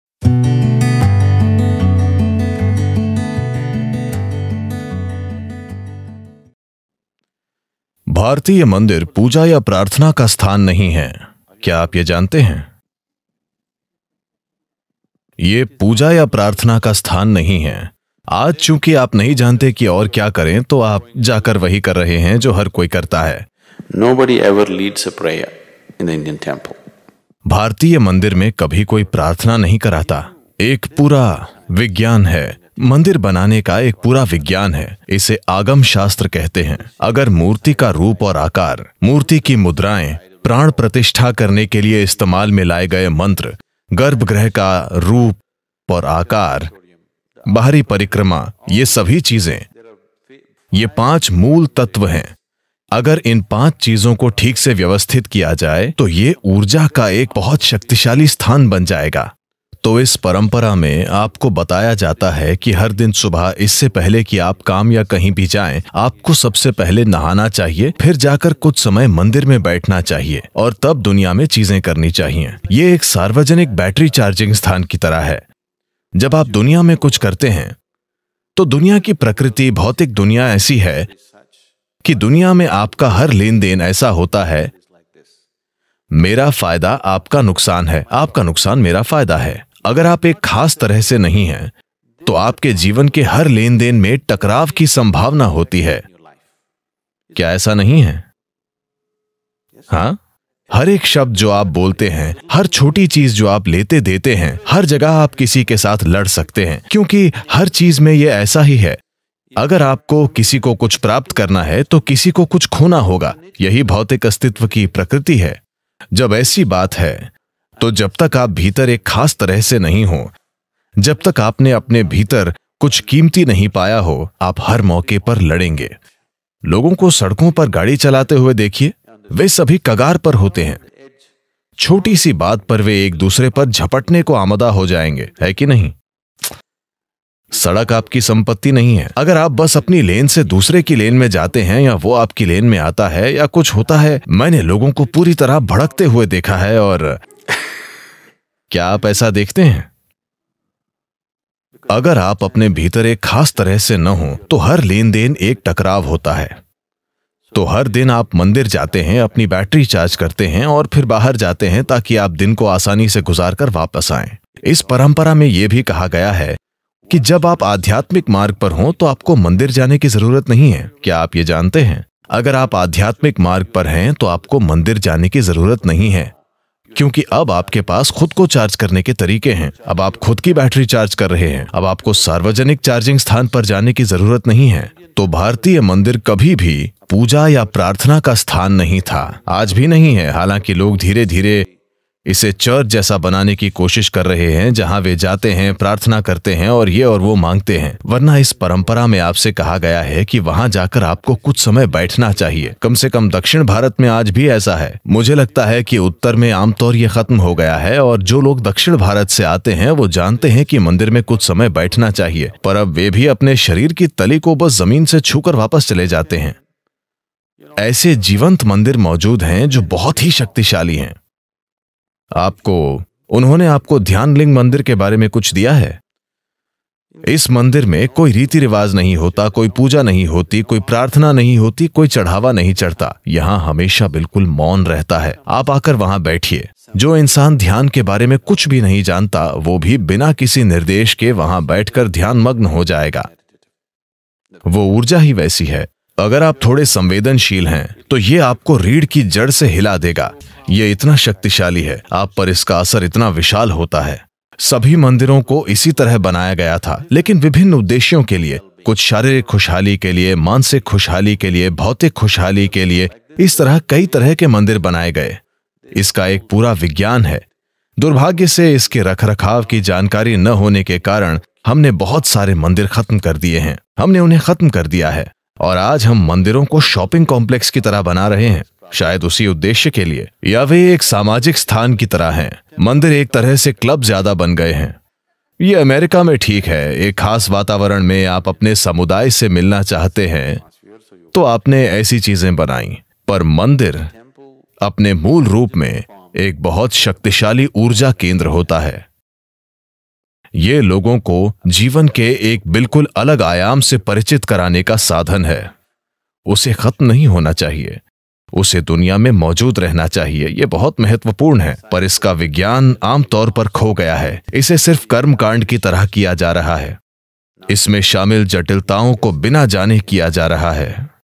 कृपया मंदिर में प्रार्थना न करें! | Sadhguru Hindi
सद्गुरु बता रहे हैं कि भारतीय मंदिर प्रार्थना या पूजा के लिए डिज़ाइन नहीं किए गए थे, बल्कि उन्हें एक विशेष विज्ञान पर आधारित शक्तिशाली ऊर्जा केंद्र के रूप में बनाया गया था। इस विज्ञान को आगम शास्त्र के नाम से जाना जाता है।उम्मीद करते है की आप सब इस विडीओ को ध्यान से सुनेगे और अपने अपने विचार सोच समझ कर विवेक के साथ हमें कॉमेंट बॉक्स में भेजेंगे!